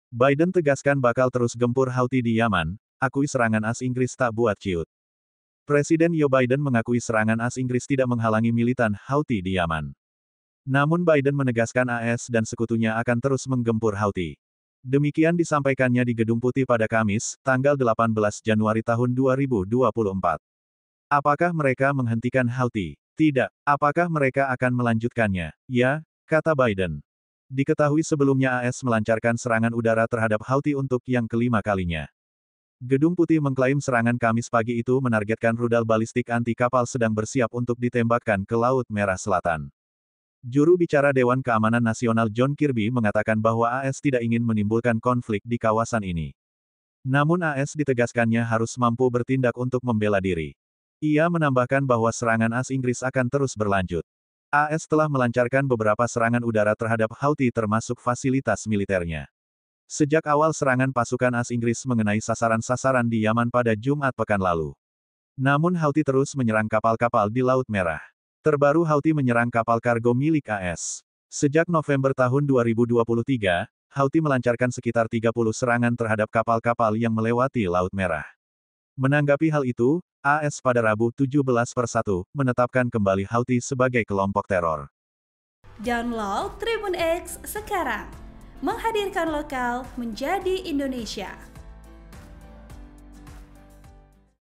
Biden Tegaskan Bakal Terus Gempur Houthi di Yaman, Akui Serangan AS-Inggris Tak Buat Ciut
VP ren

Surya.co.id

Presiden Joe Biden mengakui serangan AS-Inggris tidak menghalangi militan Houthi di Yaman.

Namun Biden menegaskan AS dan sekutunya akan terus menggempur Houthi.

Demikian disampaikannya di Gedung Putih pada Kamis (18/1/2024).

"Apakah mereka menghentikan Houthi? Tidak. Apakah mereka akan melanjutkannya? Ya," kata Biden.

Diketahui sebelumnya AS melancarkan serangan udara terhadap Houthi untuk yang kelima kalinya.

Gedung Putih mengklaim serangan Kamis pagi itu menargetkan rudal balistik anti-kapal sedang bersiap untuk ditembakkan ke Laut Merah selatan.

Juru Bicara Dewan Keamanan Nasional John Kirby mengatakan bahwa AS tidak ingin menimbulkan konflik di kawasan ini.

Namun AS ditegaskannya harus mampu bertindak untuk membela diri.

Ia menambahkan bahwa serangan AS-Inggris akan terus berlanjut.

AS telah melancarkan beberapa serangan udara terhadap Houthi termasuk fasilitas militernya.

Sejak awal serangan pasukan AS-Inggris mengenai sasaran-sasaran di Yaman pada Jumat pekan lalu.

Namun Houthi terus menyerang kapal-kapal di laut Merah.

Terbaru Houthi menyerang kapal kargo milik AS.

Sejak November 2023, Houthi melancarkan sekitar 30 serangan terhadap kapal-kapal yang melewati Laut Merah.

Menanggapi hal itu, AS pada Rabu (17/1) menetapkan kembali Houthi sebagai kelompok teror.